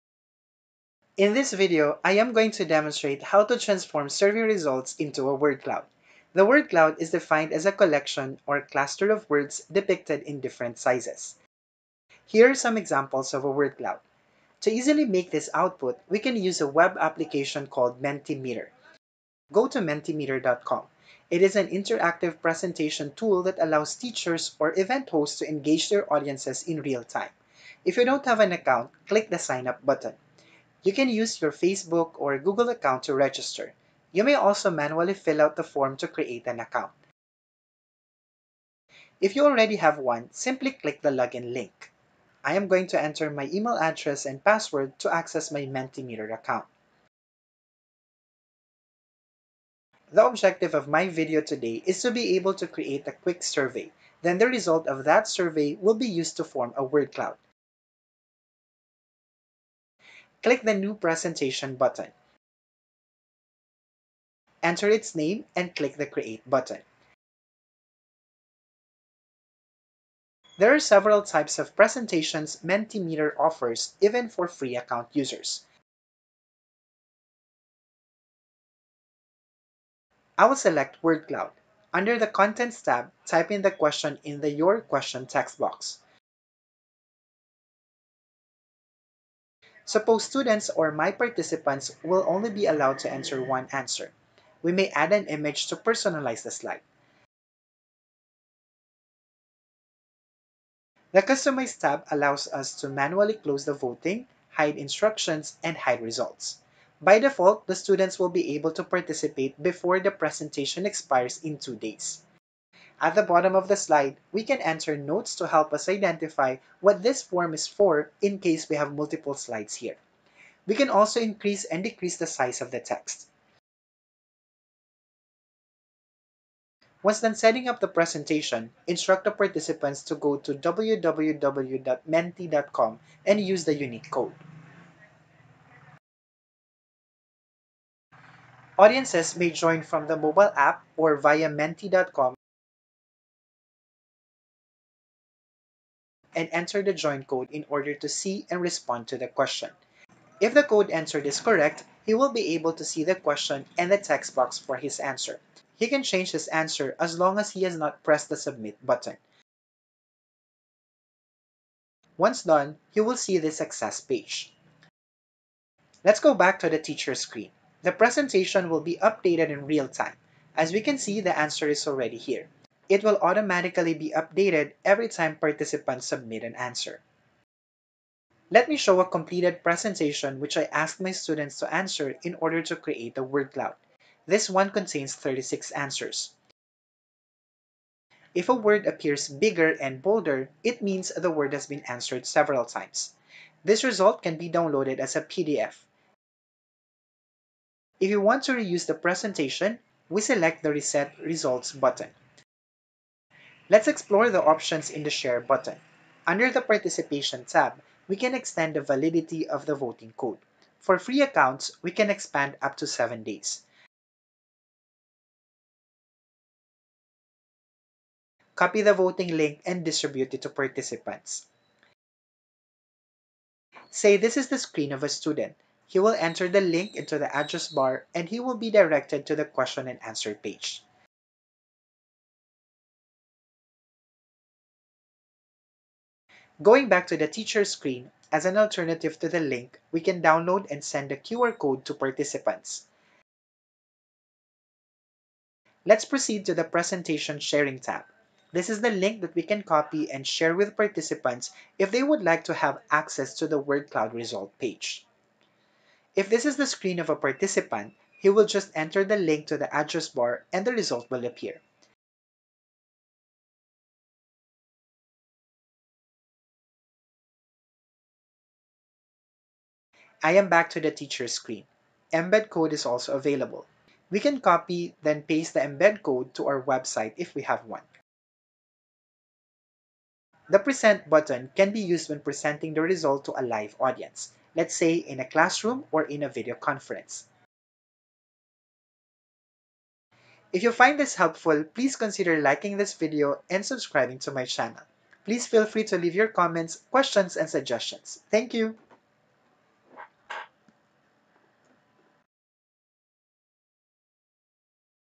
How to Transform Survey Results into a Word Cloud in Mentimeter
In this video, I will explain and demonstrate how we can transform survey results into a word cloud using @Mentimeter.